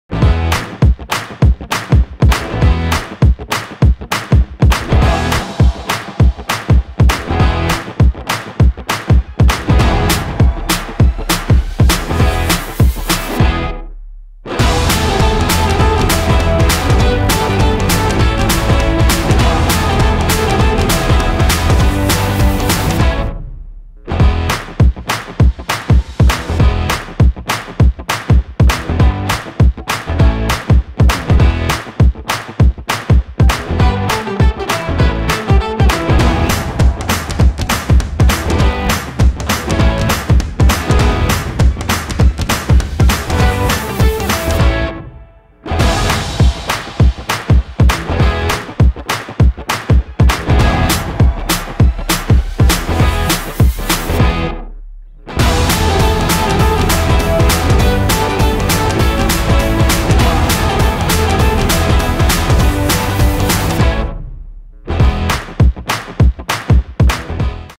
Hess Building Capacity and Reducing Emissions ENCS2: Best-in-Class Design and Build in the Bakken
Hess continues to capture more natural gas and reduce emissions in the Bakken with the construction of this next generation compressor station – delivered safely, on time and on budget.